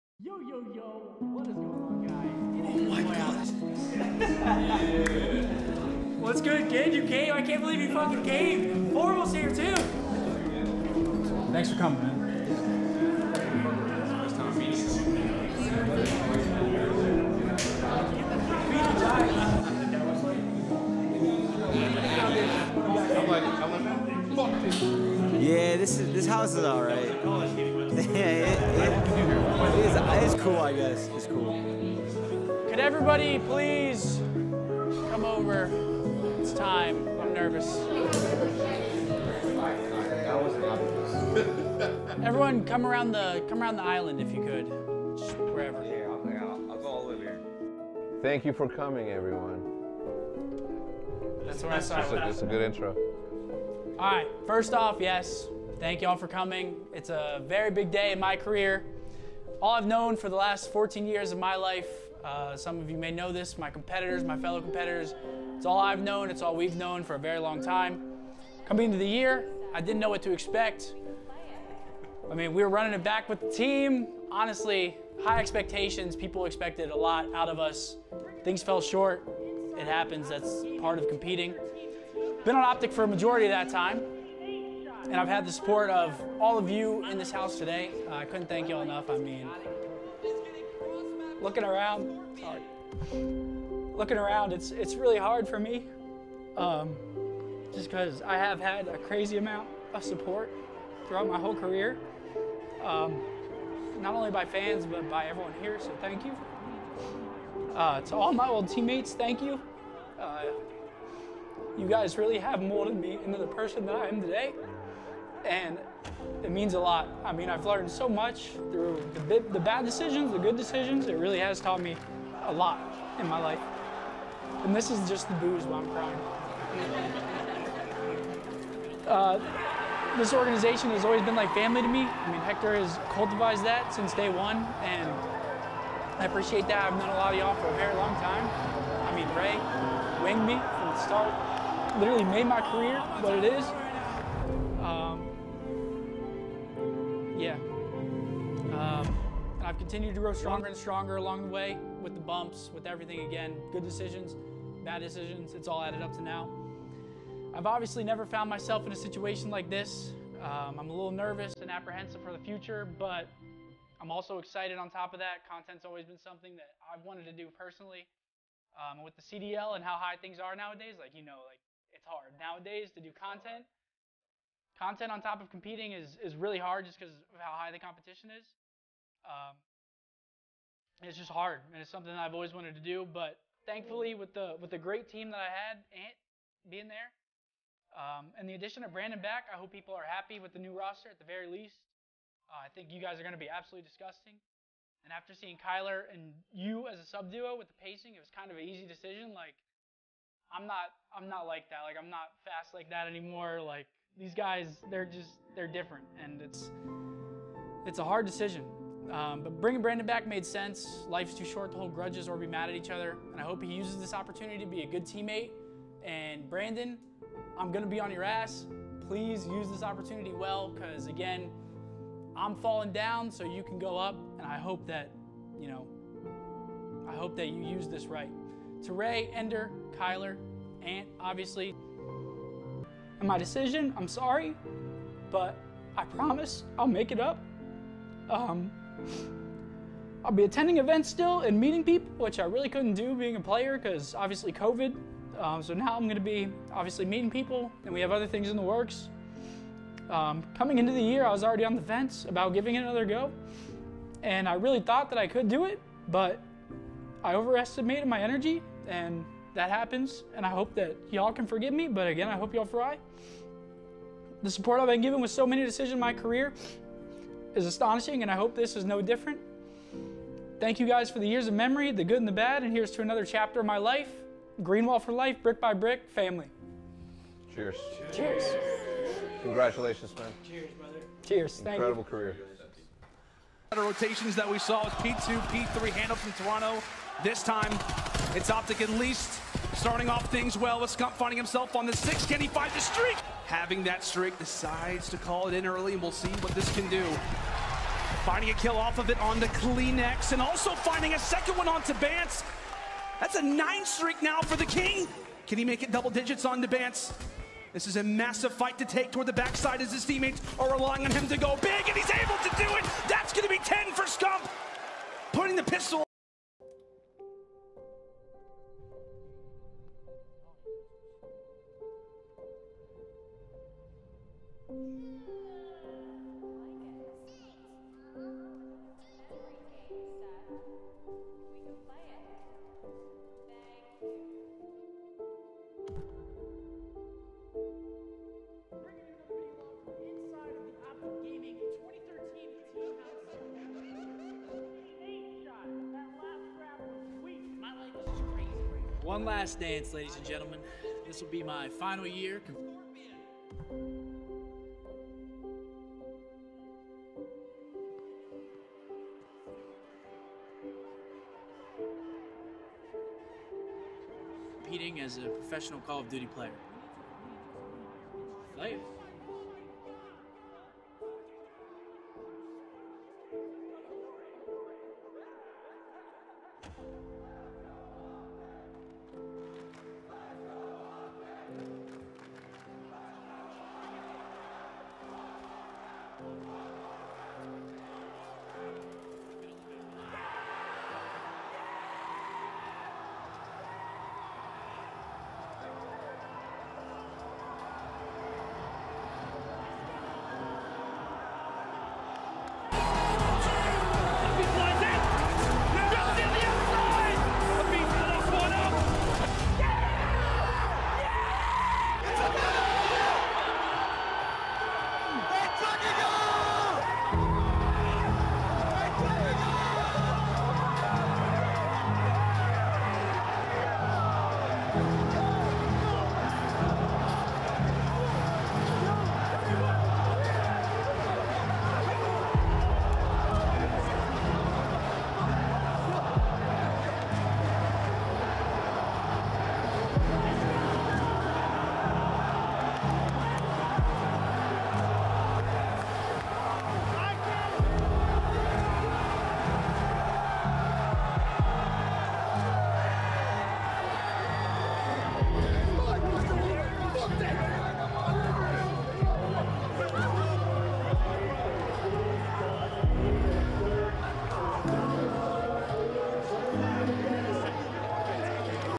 Thank you Scump!! Epic Fan edit mash up of retirement speech and clips.
Thank you Scump. Mash a video together tell what ya'll think.